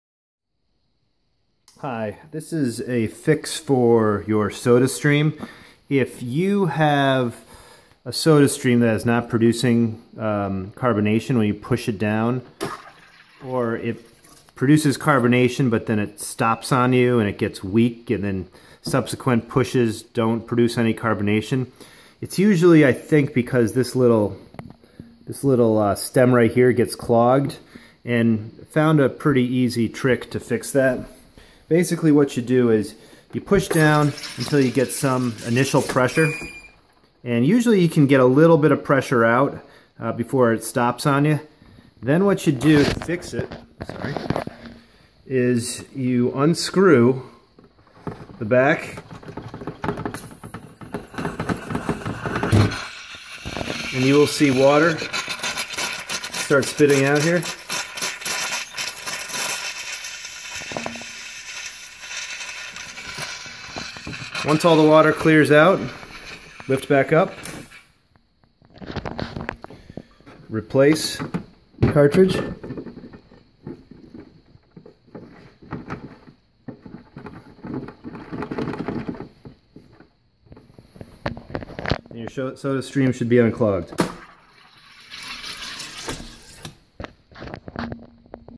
Soda Stream FIX!
For anyone who can't go a day without your Soda Stream, here is a quick fix for weak carbonation.